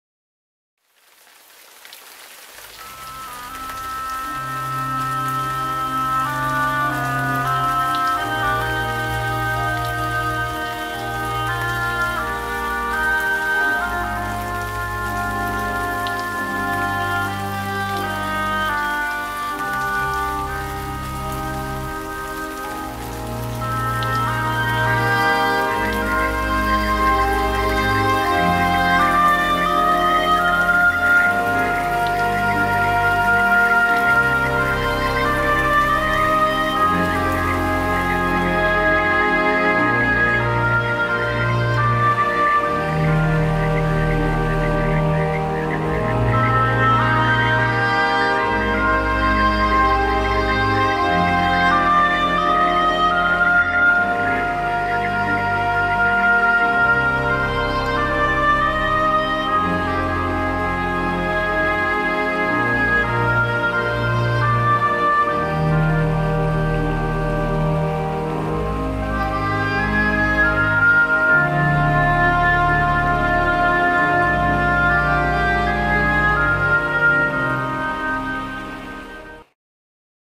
Summer-Rain Relaxing-Sound
Nature Impressions & Classic Music
Langwuhr-Weiher bei Eintürnen
Video-Realisation: Bernhard Kling
Oboe: Alexandra Freiin von Beaulieu-Marconnay